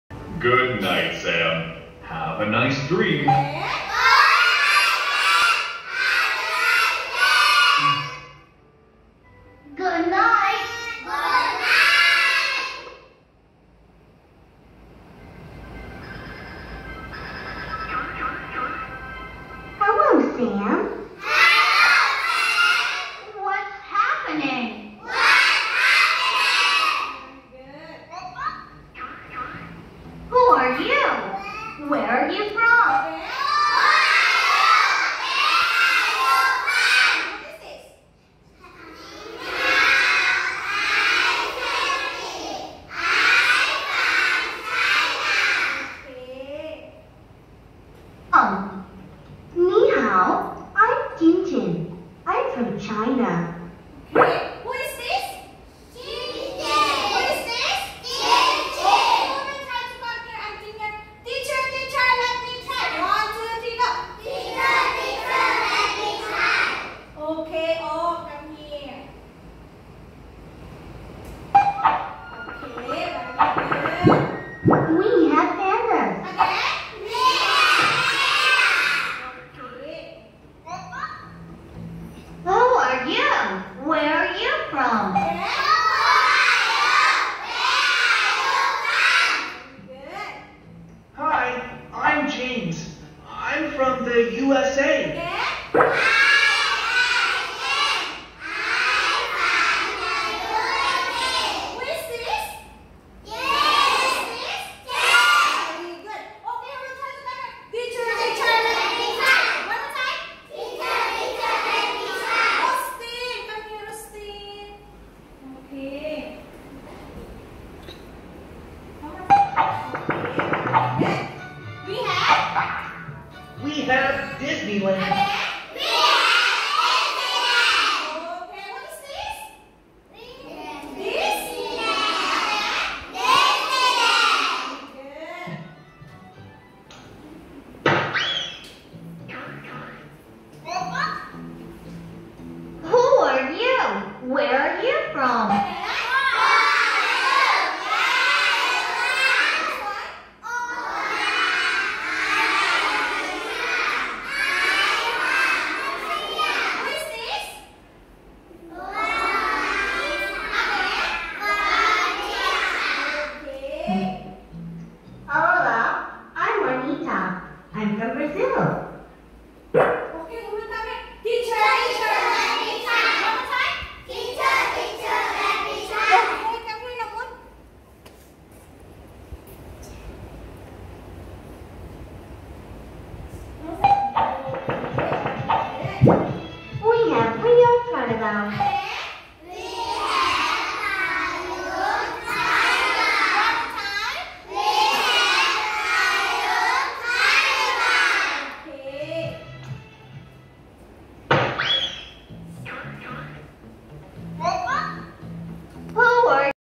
English “Kids Brown Wings” Class @ โรงเรียนอนุบาลธรรมรัตน์# K.3 by ศูนย์นวัตกรรมการศึกษา Siam i-Edu
✨English “Kids Brown Wings” Class at Thummarat Kindergarten School✨ คลาสเรียนภาษาอังกฤษแสนสนุกที่โรงเรียนอนุบาลธรรมรัตน์ อนุบาล 3
🎠🌎Doll's World🌎🎠 เรียนรู้เกี่ยวกับประเทศต่างๆ ชื่อประเทศ วัฒนธรรมเด่นๆ สัญลักษณ์ของแต่ละประเทศ สกุลเงิน ฯลฯ รวมทั้งประโยคคำถามและคำตอบ ผ่านกิจกรรมการเรียน เพลง นิทาน เกมส์ งานประดิษฐ์ การทำแบบฝึกหัดที่สอดคล้องกับบทเรียน .
🎠🌎Doll's World🌎🎠
•Countries •Nationality
•Learning about the names of countries
•Asking & answering about one’s nationality
คำศัพท์ เช่น
รูปแบบประโยค เช่น
•Where are you from?
•I’m from China. We have pandas.
•I’m English. We have Big Ben.
•Konnichiwa! I’m from Japan.
•Hello! I’m from U.S.A.
•Jump like a Kangaroo.
การออกเสียงโฟนิค เช่น
Oo : October, octopus, omelet, ox
Uu : umbrella, uncle, up, upset
และอื่นๆ อีกมากมาย ฯลฯ

👼Kids Brown Wings👼  Integrated English Program for Kindergarten โปรแกรมภาษาอังกฤษบูรณาการทางการศึกษา Thinking, คณิตศาสตร์, วิทยาศาสตร์, ดนตรี, สังคม, ศิลปะ, การออกเสียง Phonics ฯลฯ 4 ทักษะการเรียนรู้ ฟัง พูด อ่าน เขียน ในหนึ่งโปรแกรม โดยมหาวิทยาลัยบราวน์ (Brown University) U.S.A
สนุก✨สร้างสรรค์✨เสริมสร้างจินตนาการ✨
By ศูนย์นวัตกรรมการศึกษา Siam i-Edu Center English Program